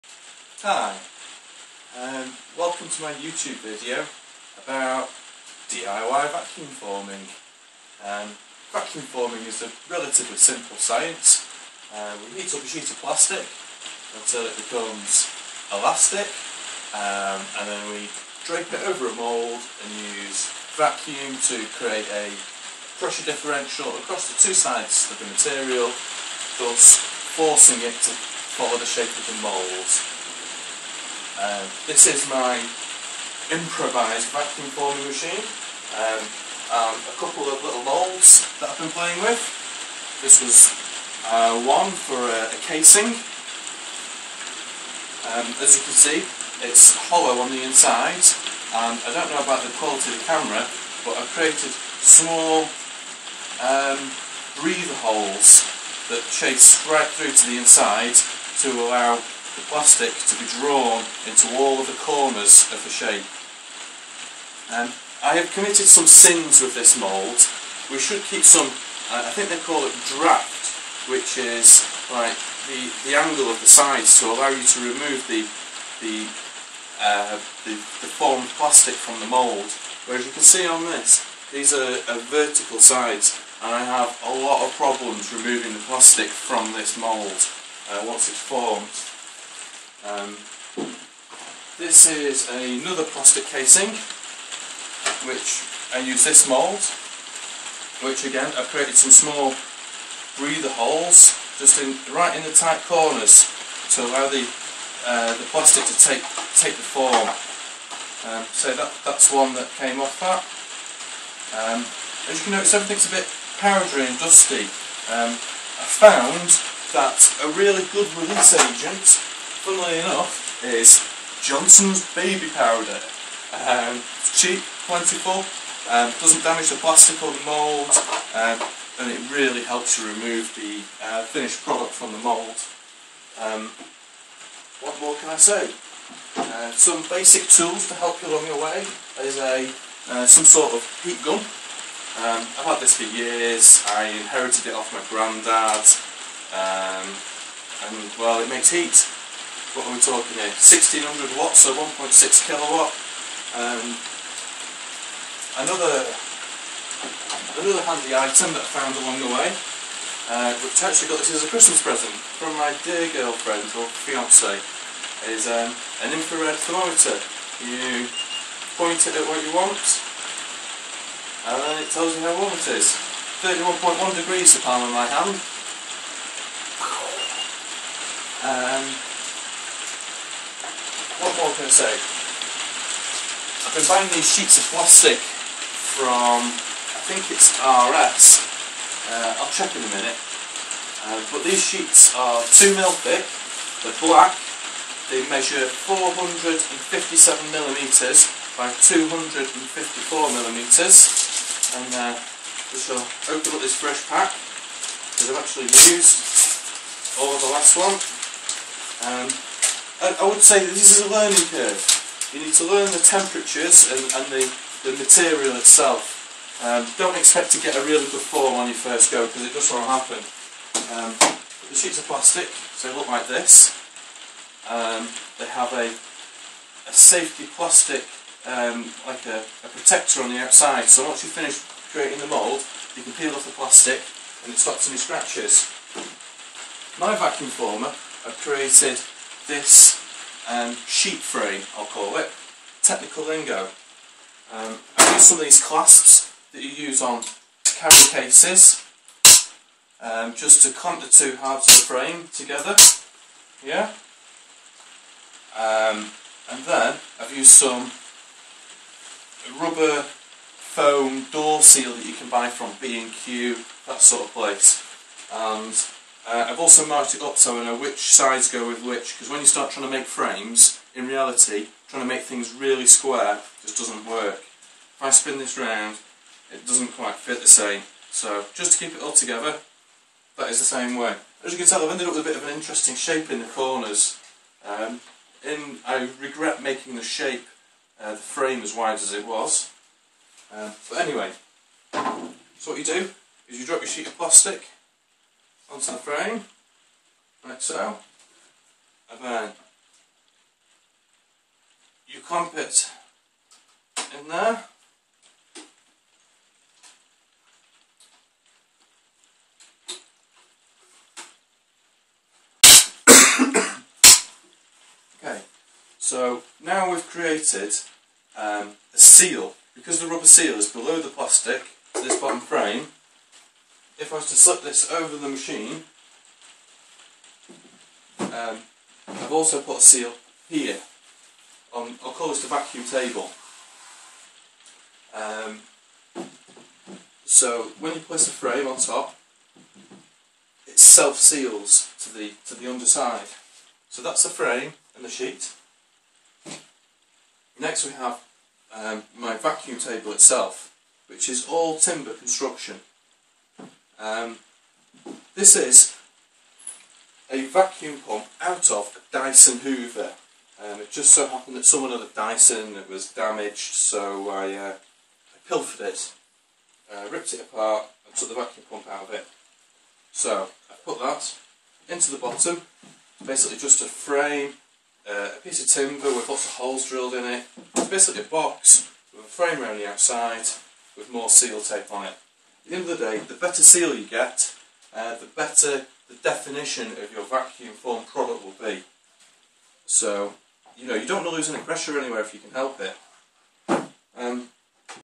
Part 1 - Intro - DIY Vacuum Forming, build a machine for under £20 ABS PETG HIPS
Please excuse the sound, it is rain boucing off the roof.

I am not a video blogger, actor or editor - this is the full uncut video!!